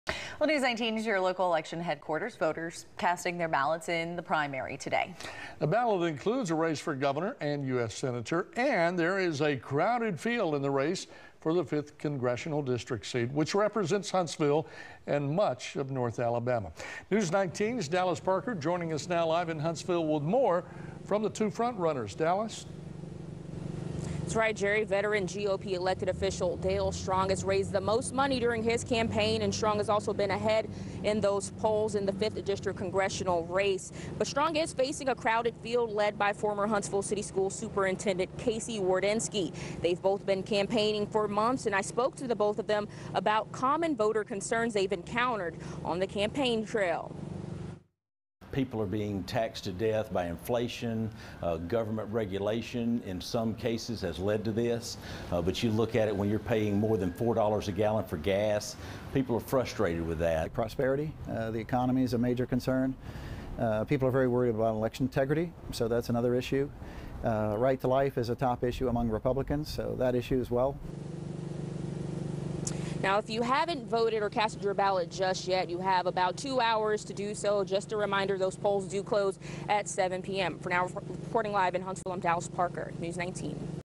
Congressional District 5 Race
Alabama's Primary election ballot includes a race for Governor and US Senator and there is a crowded field in the race for the 5th congressional district seat, which represents Huntsville and much of North Alabama.